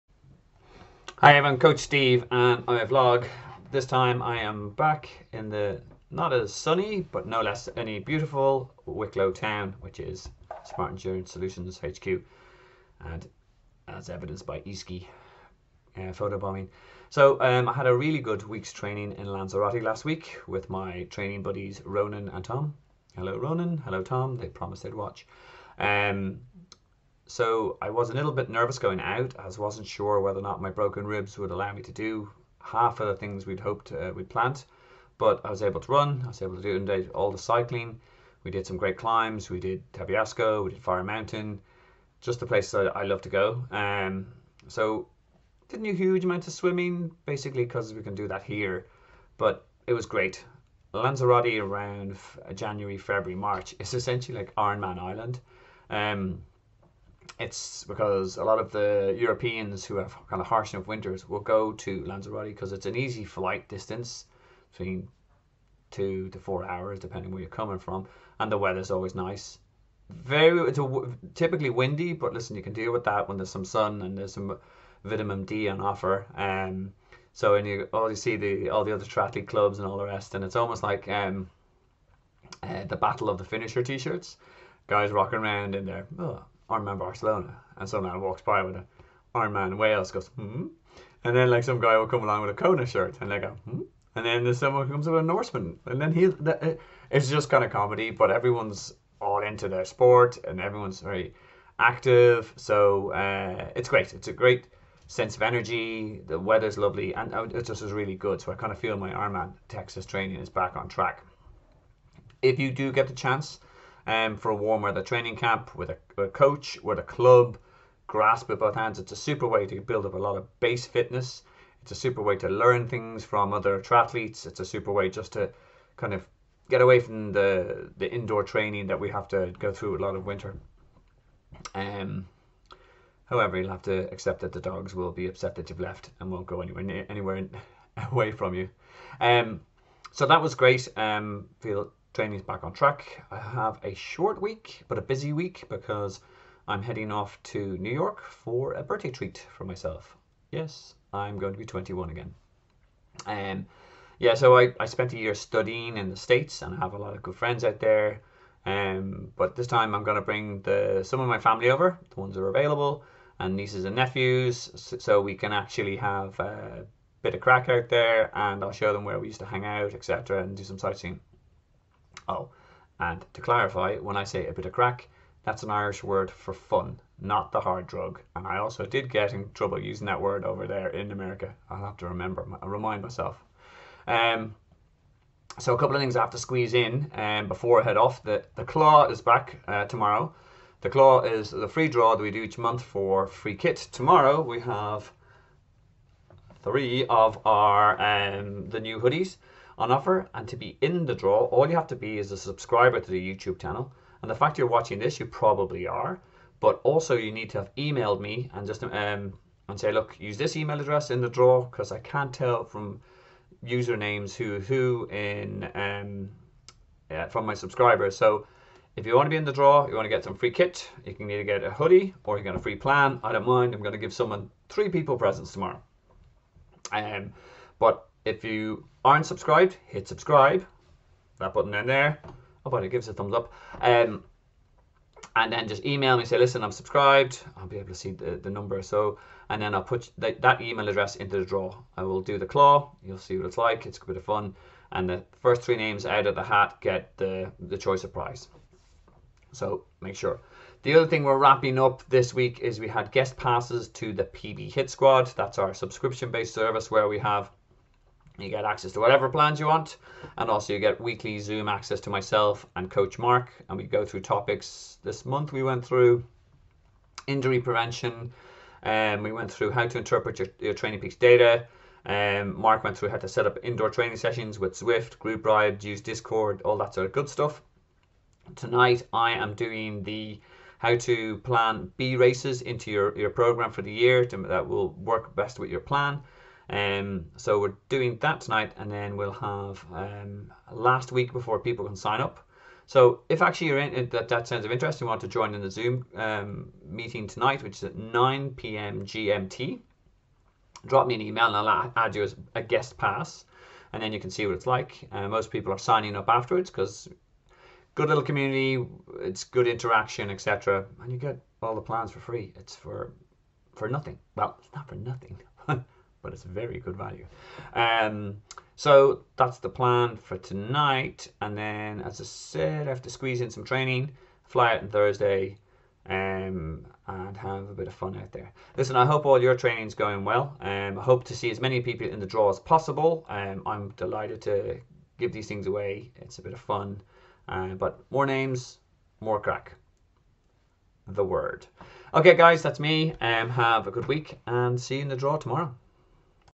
Coach Steve Vlog - Back from Lanza and return of the claw
Coach Steve Vlog
- Post Lanza thoughts
- NYC plans
- Last min guest passes to a PB Hit Squad session
- Return of the claw